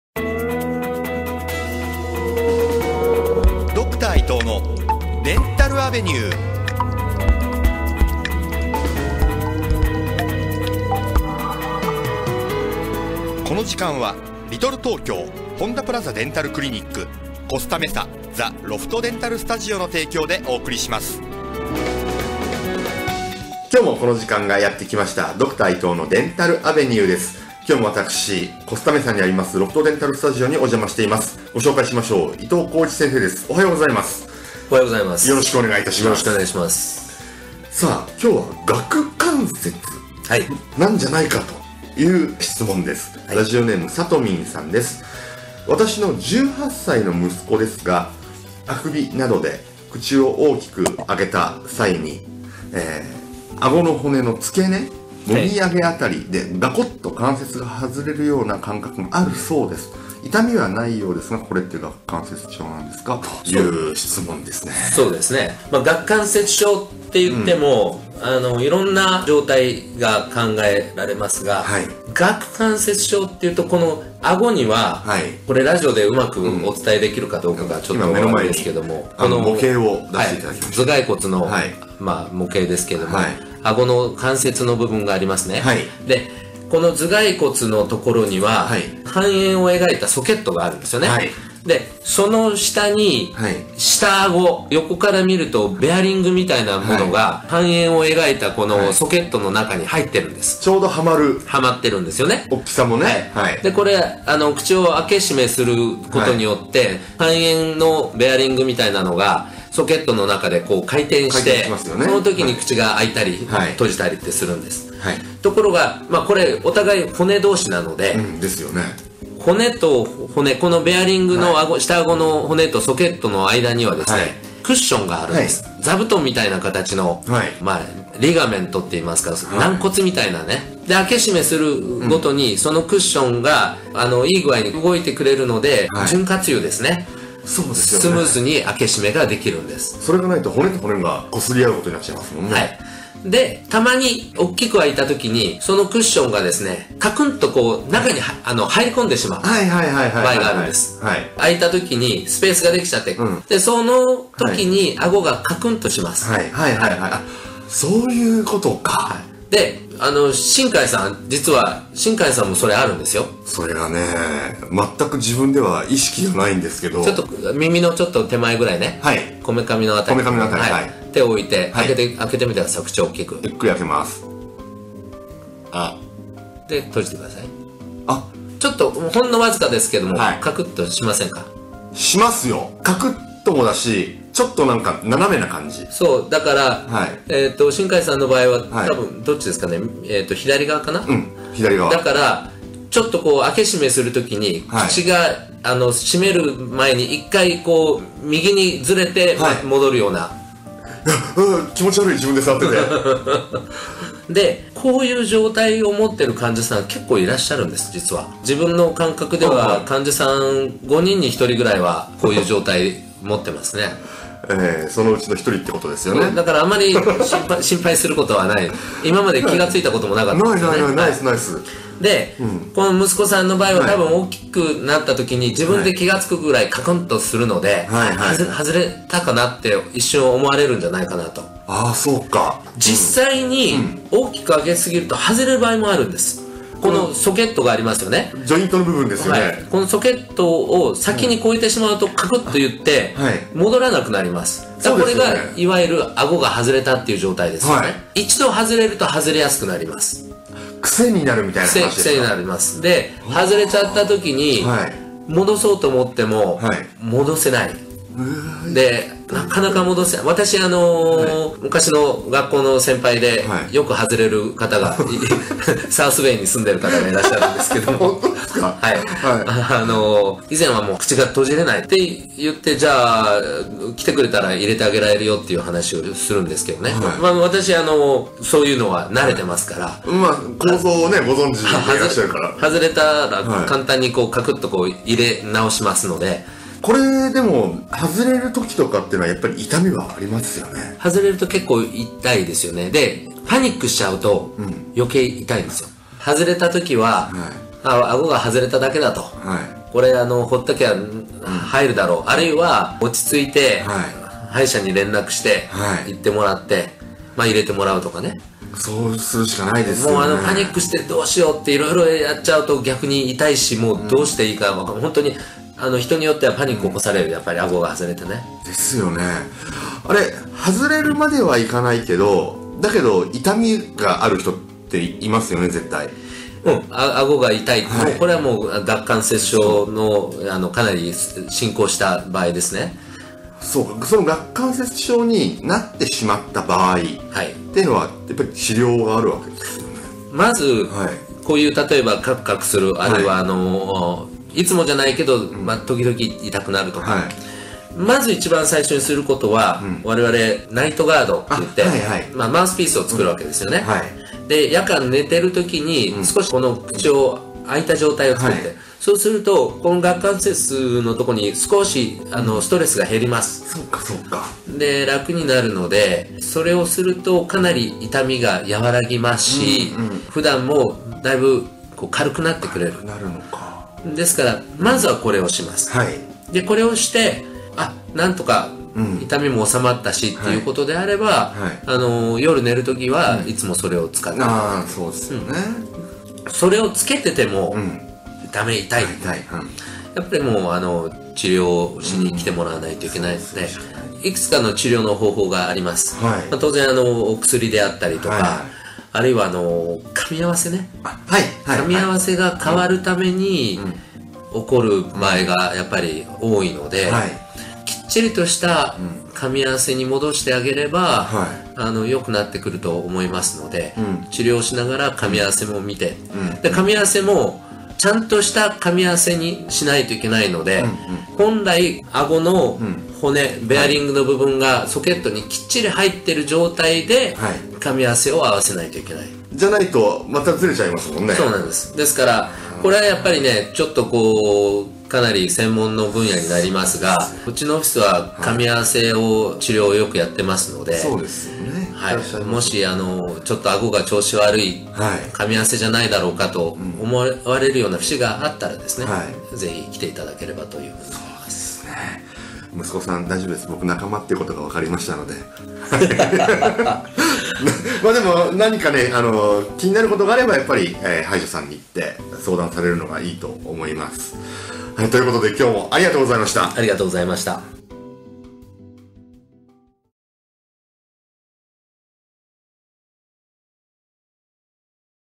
「顎関節症と治療について」｜６月６日 Ito's Dental Ave.
こんにちは！
今日は伊藤先生から顎関節症について教えていただきます♪